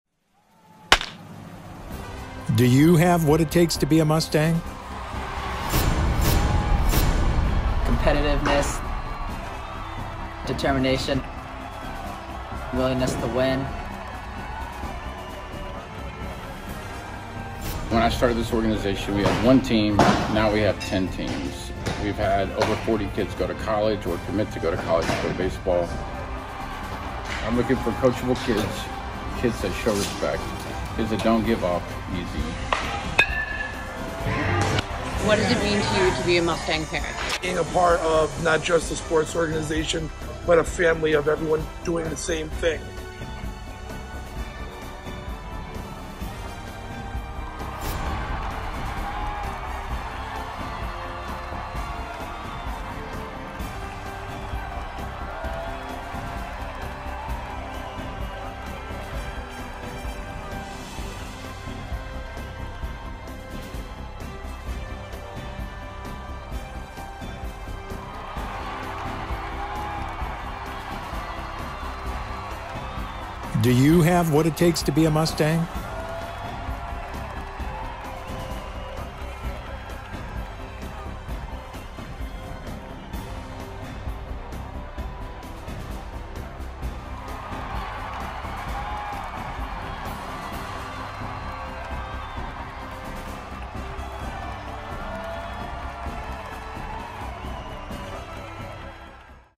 Come see if you have what it takes to be a Mustang!
You won’t know unless you try!

The Northern CT Mustangs are excited to announce tryouts for the Spring 2020 season! As Mustangs, we are committed to the pursuit of building a unique travel baseball program grounded in a positive and trusting environment.

The selected players will experience a fun, yet extremely competitive environment. Our coaching staff teaches proper mechanics, and good baseball IQ.  We are a family that consists of players, parents, and coaches that understand the importance of humility, teamwork, and sacrifice. Through practice we will learn about accountability, discipline, and being the best version of us that we can be. The outcome of the game will allow us to celebrate the win, or learn from our loss, both as young men of character.

If you wish to be considered for the Spring 2020 teams, you must attend tryouts. The dates and times are listed below as well as the link for registration.

Thank you, good luck to everyone at tryouts and GO MUSTANGS!

Ages 9U - 12U

November 9 & 10, 1pm-3pm
Ages 13U - 15U

November 16 & 17, 1pm-3pm
Ages 16 - 18 by appointment only